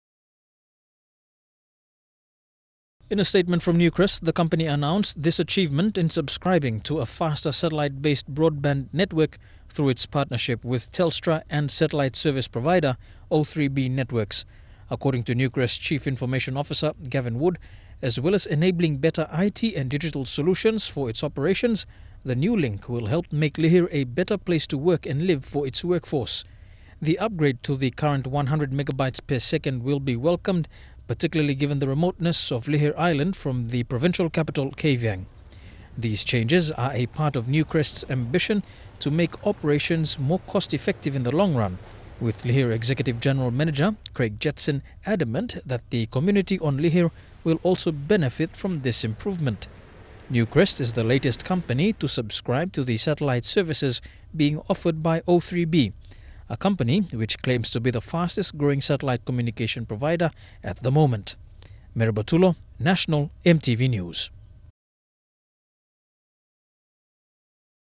Lihir first with high-speed broadband link
The expectation of fast and efficient telecommunication services within a mine environment is crucial.

With this in mind, Newcrest, operators of Lihir Mine, have partnered with Australian telco, Telstra, and O3b Networks to deliver a high-speed broadband link linking the mine to the rest of the world.

Lihir has become the first mine within the Southern hemisphere to implement this technology.